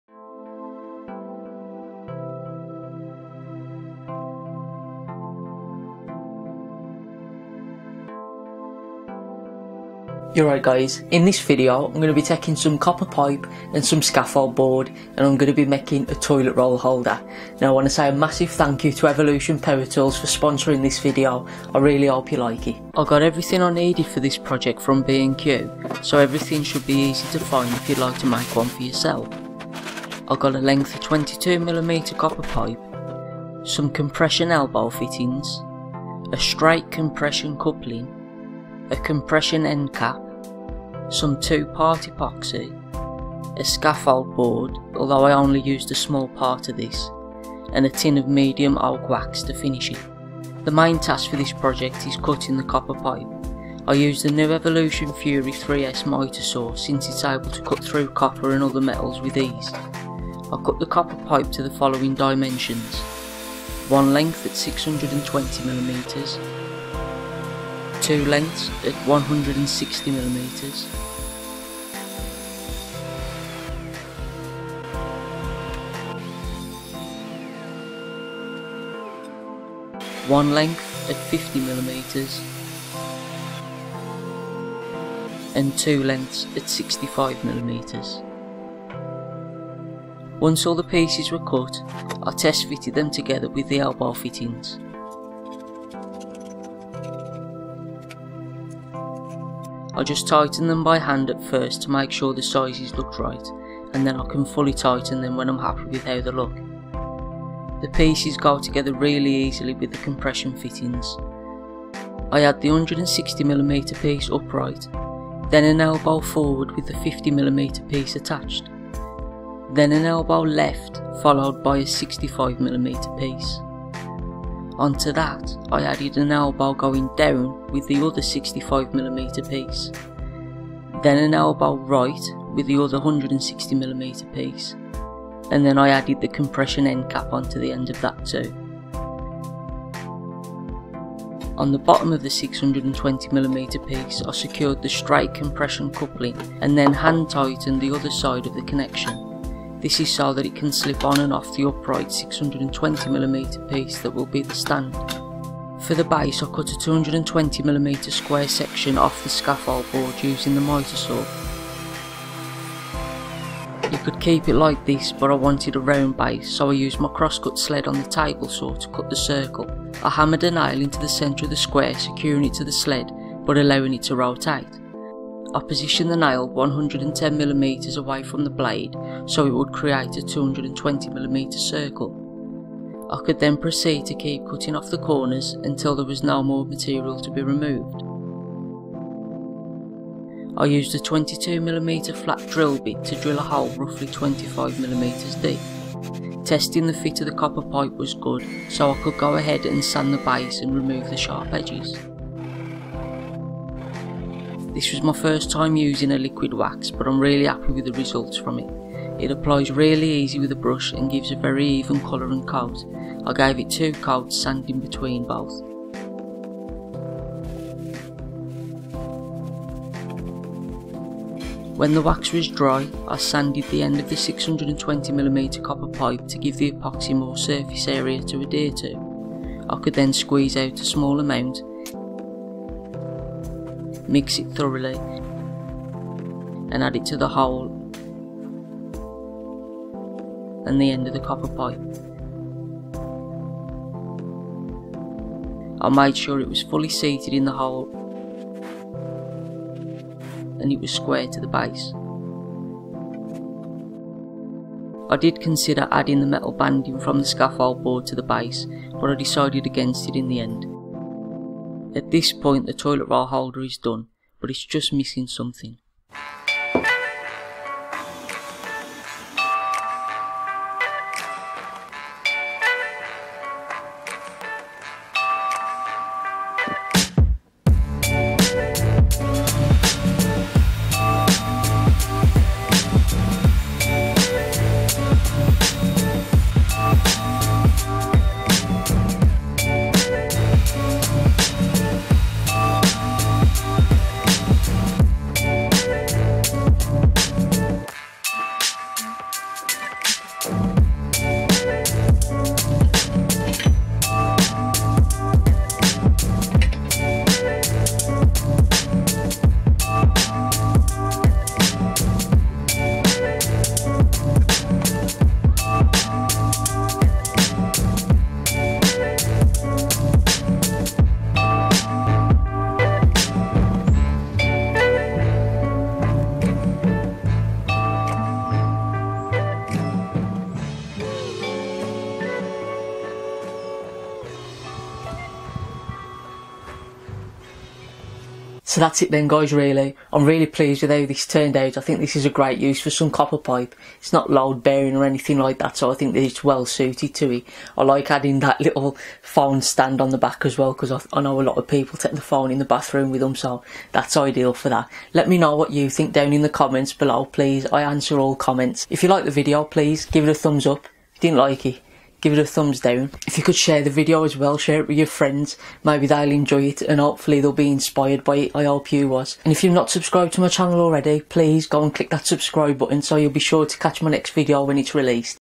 Copper Pipe Toilet Roll Holder - Quick & Easy Project
After using copper pipe for my last project I had an idea for this toilet roll holder. I hope you like it :)
This project is really simple to make and there is loads of opportunities to make changes and really make the idea or design your own. I hope you are inspired by it.